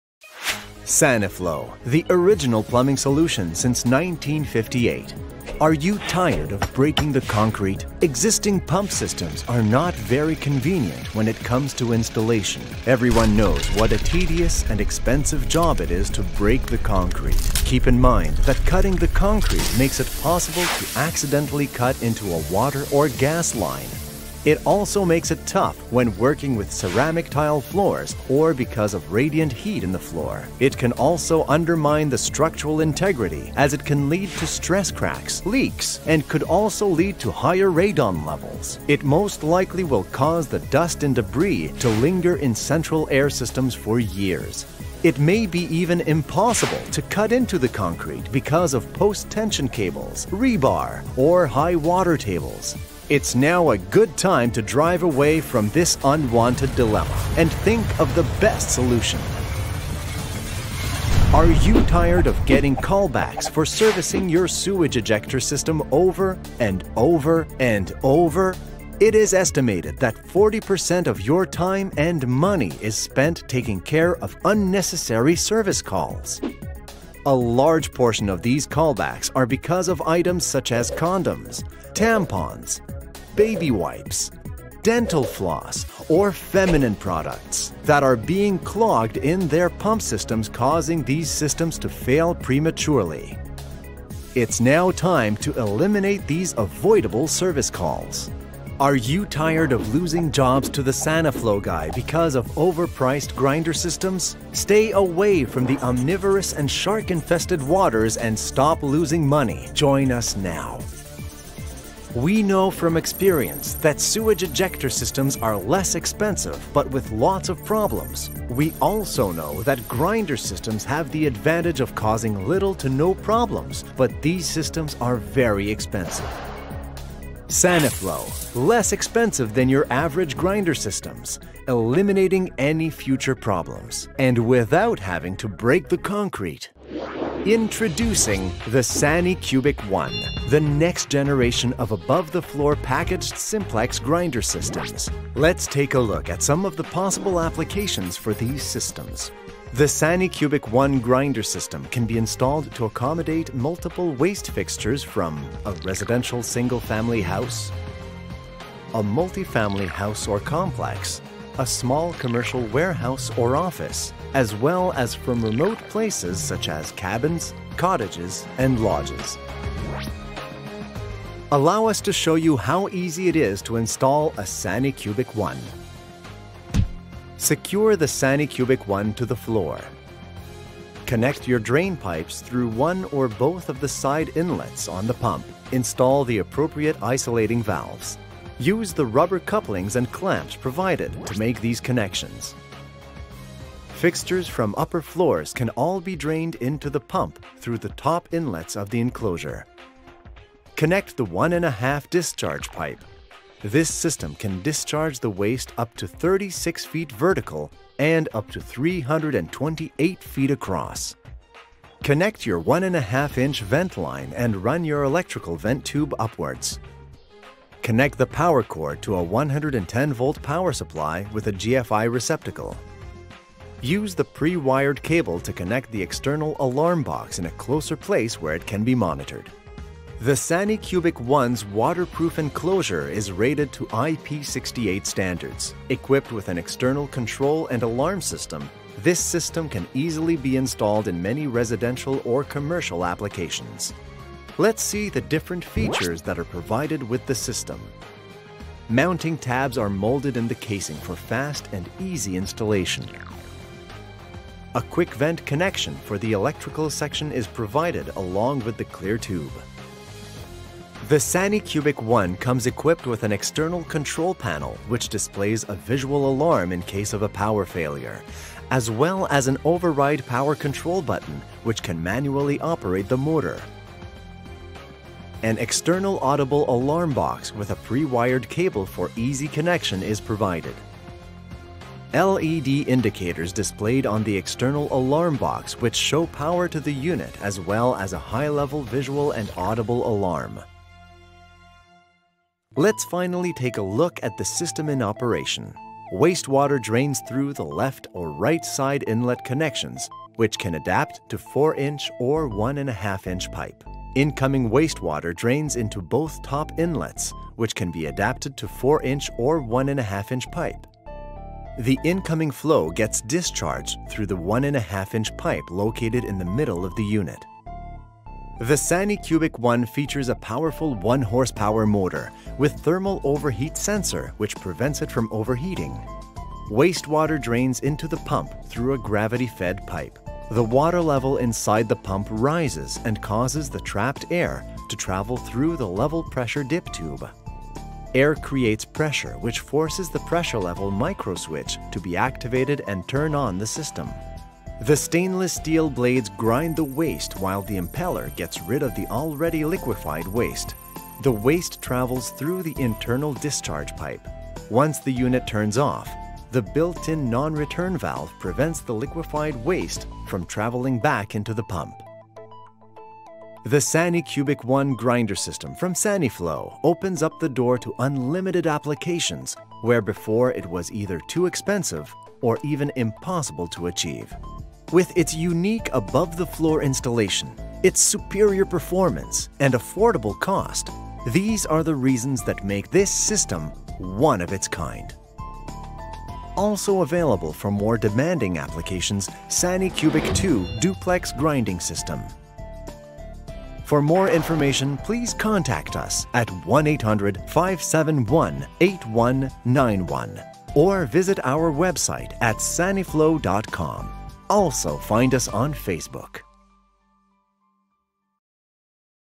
SFA - Sanicubic 1 IP 68 new model
Maybe you’ve heard that the Sanicubic 1 and Sanicubic 2? They are Saniflo’s heavy-duty lift stations ideal for handling the sewage of multiple fixtures from an entire modular, residential and industrial building.

This IP68 rated lift station eliminates any potential water damage to the internal parts? It can be installed above the floor, inside a pit or even retrofitted into an existing sewage ejector basin.